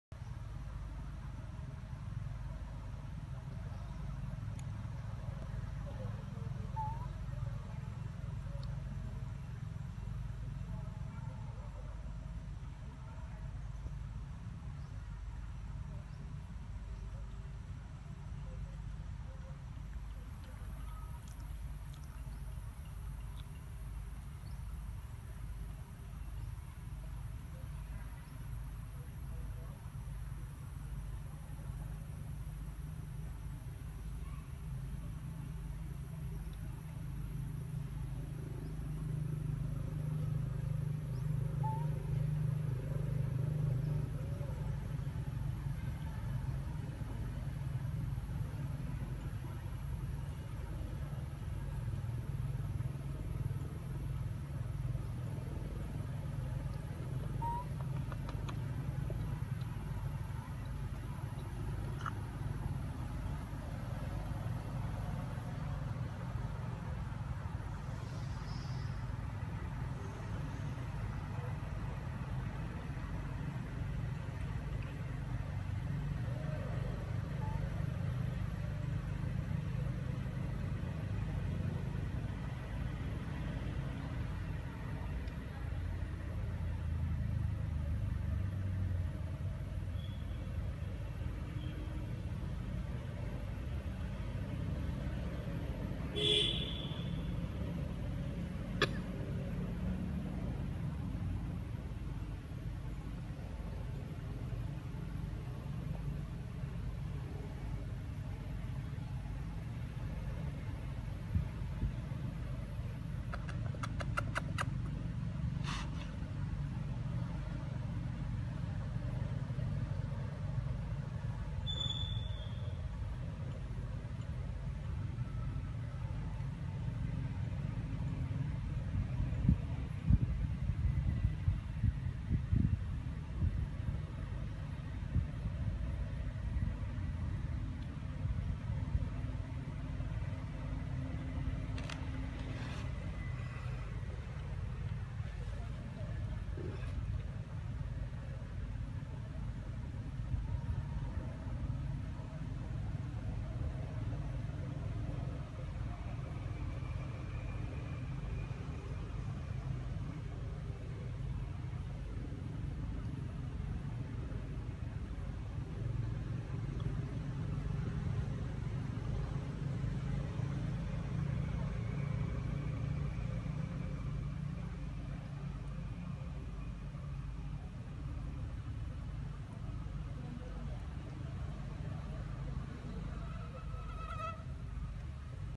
Great Milk! Jason Get Milk From Mum Jill sweetly Ep855
Amari Troops. !
Please Stay Away From Covid-19 together.
Lovely Troops that don’t leave them alone. Mum Jill With Her adorable baby Jason rest peacefully

You can watch my original videos even when you aren’t connected.